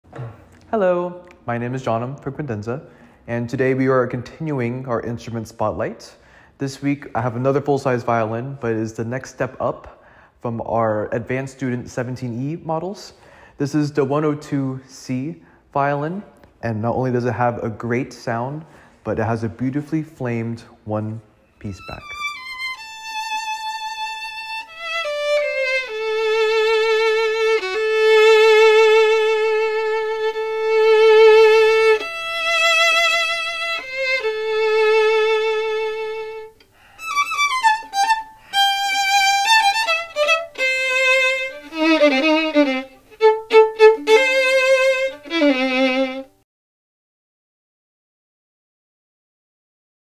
102C Violin - INSTRUMENT SPOTLIGHT
On January 2023, we featured full-size violin models at Cadenza Violins.  In this video, we show off our 102C violins, the next full-size student model up from our previous 17E’s.  Make sure to stop by our shop to hear and play our full selection of violins and other instruments!

And come see us in-person!